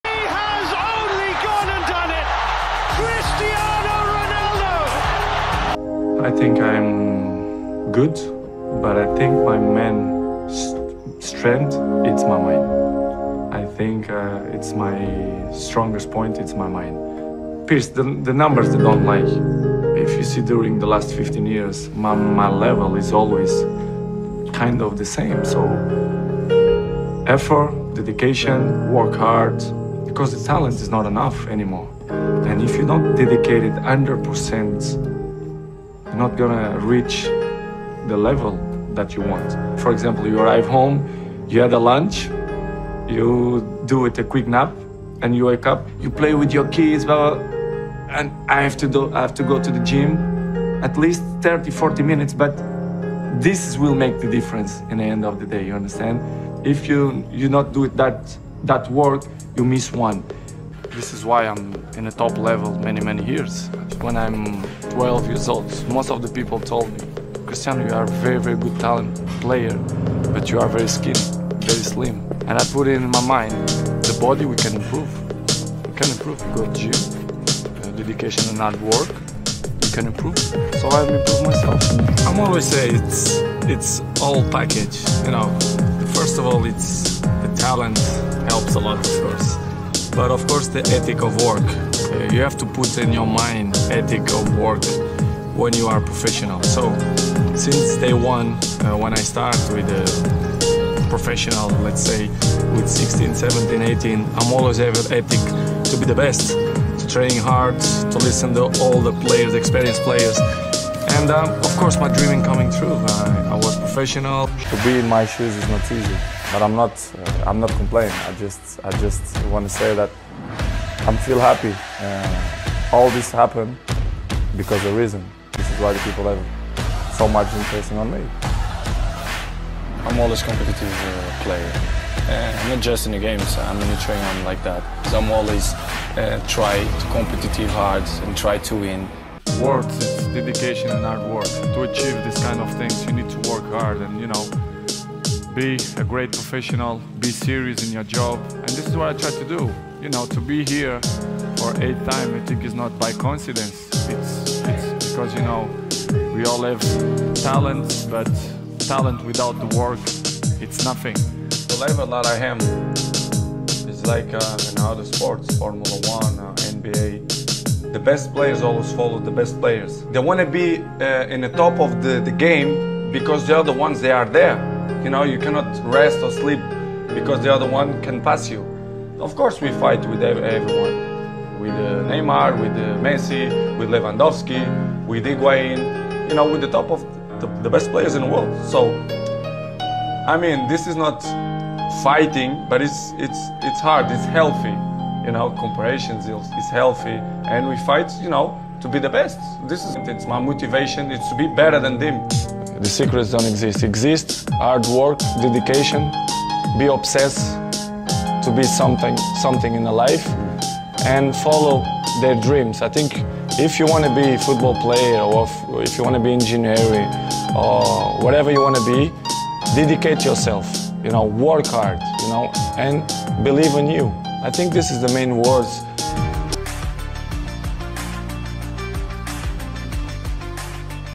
The Greatest of All Time: Cristiano Ronaldo's Untold Secret
In this video, we bring you a powerful dose of motivation featuring the legendary Cristiano Ronaldo. Prepare to be inspired as we delve into Ronaldo's journey and uncover the mindset and dedication that has made him one of the greatest footballers of all time. Join us as we explore the lessons we can learn from Ronaldo's incredible work ethic, discipline, and unwavering determination. Get ready to be motivated and discover how Ronaldo's story can positively impact your life!